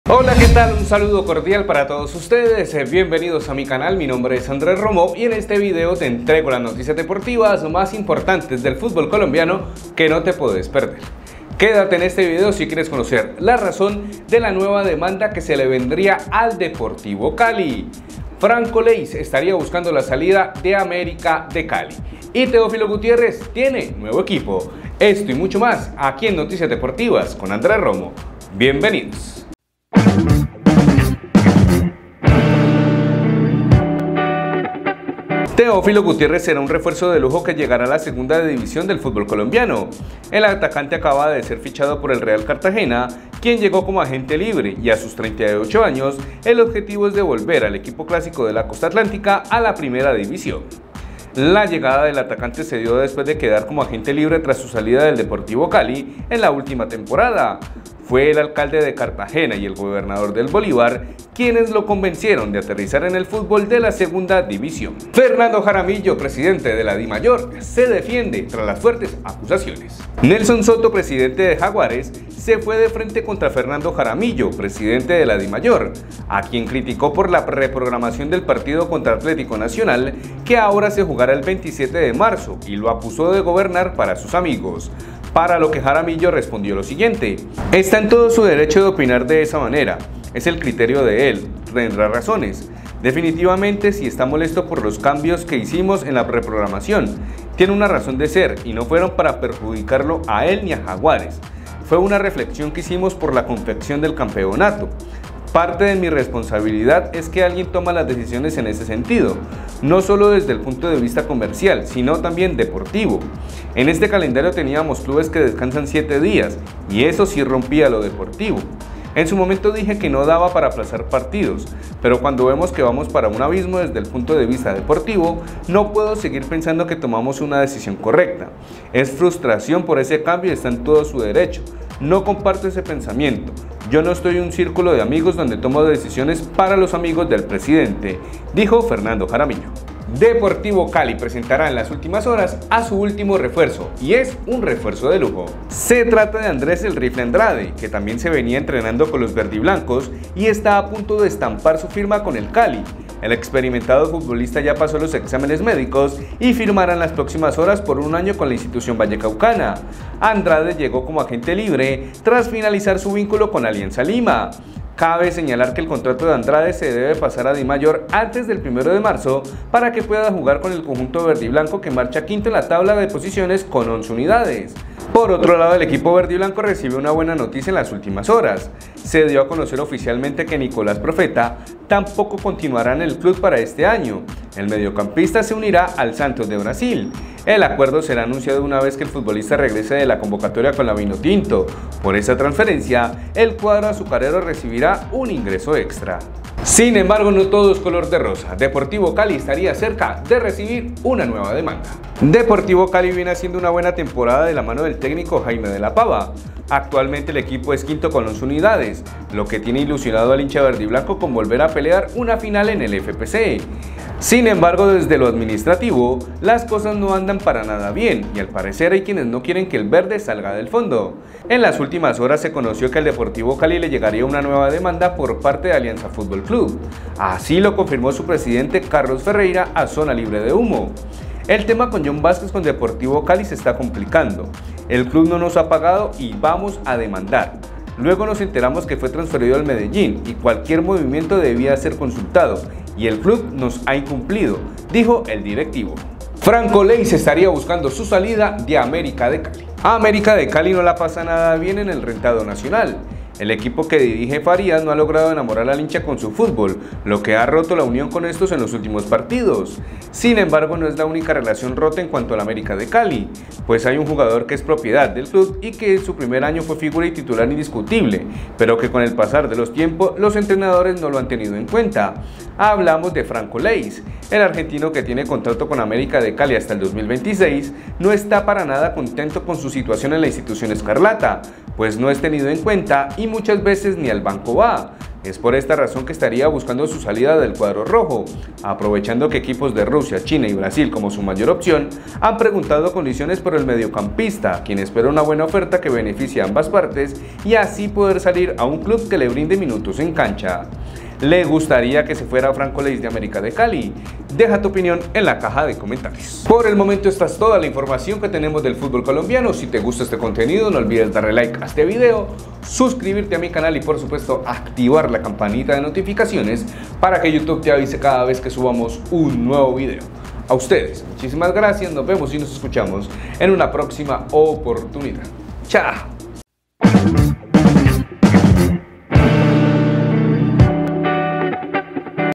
🚨 NUEVA DEMANDA PARA DEPORTIVO CALI 😱| LEYS BUSCA SU SALIDA DE AMÉRICA DE CALI ✈️
Qué tal parceros en este video te entrego las noticias deportivas más importantes del fútbolcolombiano

- Teófilo Gutiérrez se queda en el FPC. Es nuevo jugador del Real Cartagena.
- Fernando Jaramillo presidente de la Dimayor se defiende de las fuertes acusaciones.
- Deportivo Cali presentará su último refuerzo en las próximas horas.
- Presidente de Alianza FC molesto con Deportivo Cali, lo demandará.
- Franco Leys busca su salida de América de Cali.